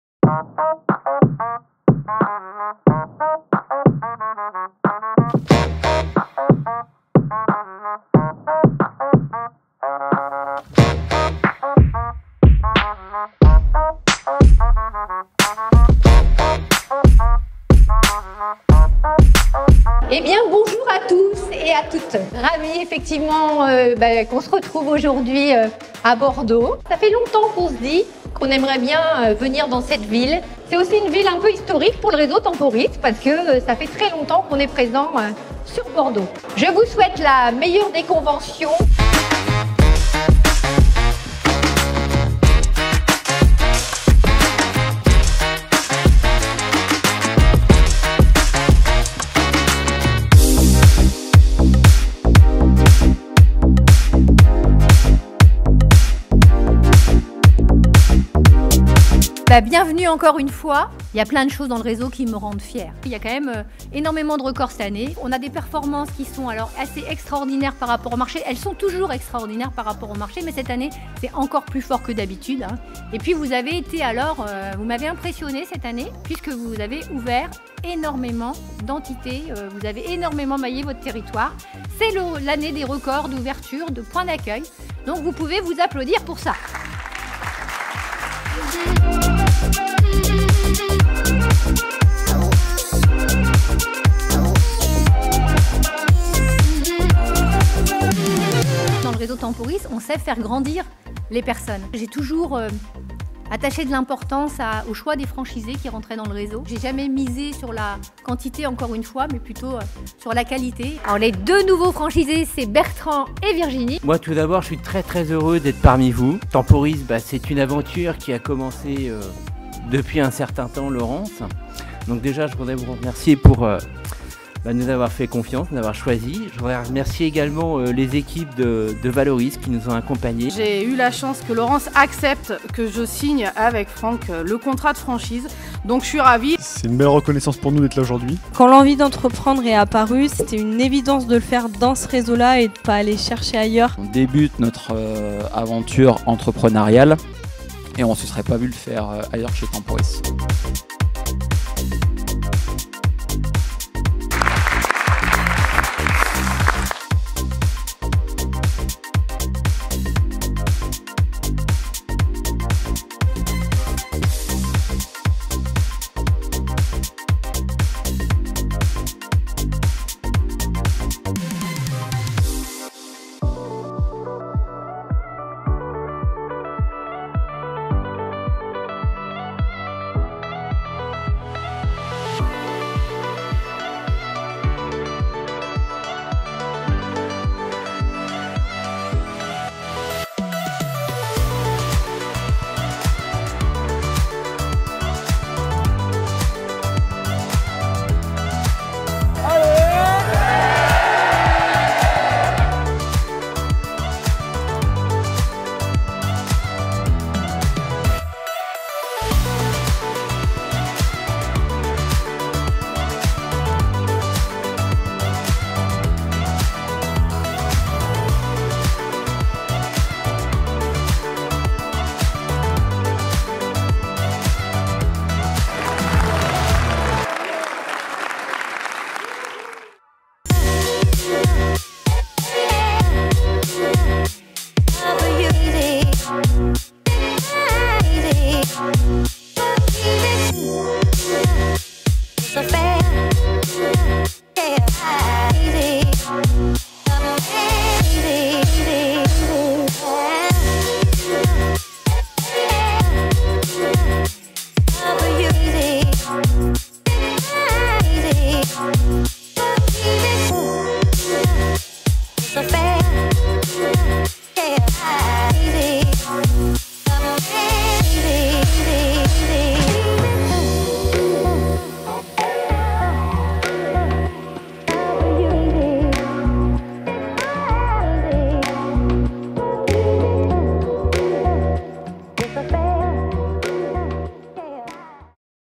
Convention Temporis 2023
Chaque année, les franchisé·es du réseau Temporis se réunissent pour passer un moment de partage d'information, d'expérience, d'activités et repartir gonflé·es à bloc pour les prochains mois !

Et pourquoi pas devenir franchisé chez Temporis ?